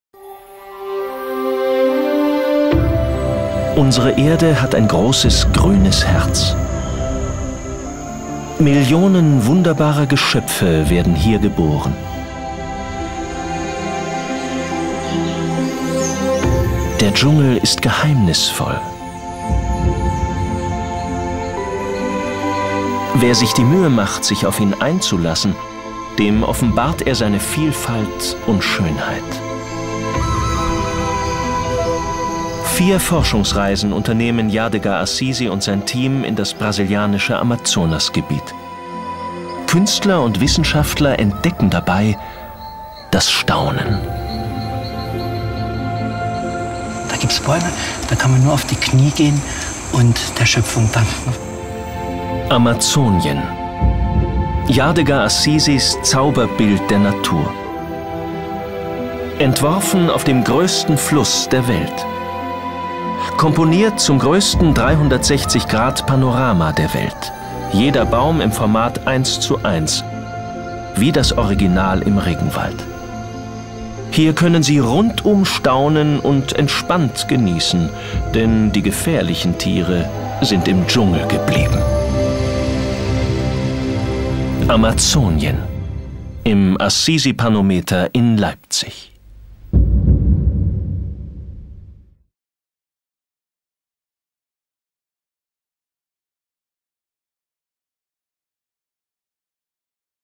Amazonien - eine Reise in den Regenwald - weltgrößtes 360°-Panorama in Leipzig
Der Künstler Yadegar Asisi zeigt ab 9. Juni 2012 im Panometer Leipzig das weltgrößte 360°-Panorama "AMAZONIEN - Yadegar Asisis Zauberbild der Natur. Das Video vermittelt einen Eindruck von der Entstehung des einzigartigen Kunstwerks.

Yadegar Asisi, an artist from Berlin, has recreated this world for us in his 360° panorama AMAZONIA. Trees growing high up into the sky, long trailing roots which seem to hang down from nowhere, a green half-light and a concert made up of the many thousands of voices of all the animals living here. On a scale of 1:1, around 106 metres long and 30 metres high, AMAZONIA reveals the complexity and beauty of the faraway Brazilian Rainforest.